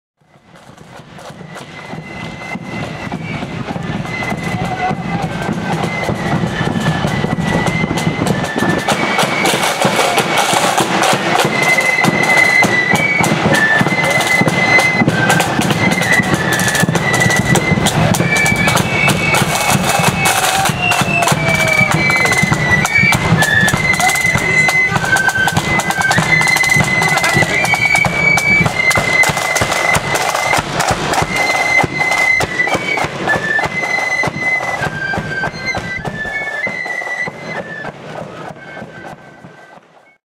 Cloughfern Young Conquerors Easter Monday Antrim 2018
Cloughfern Young Conquerors on the march with the Apprentice Boys of Derry in Antrim on Easter Monday 2nd April 2018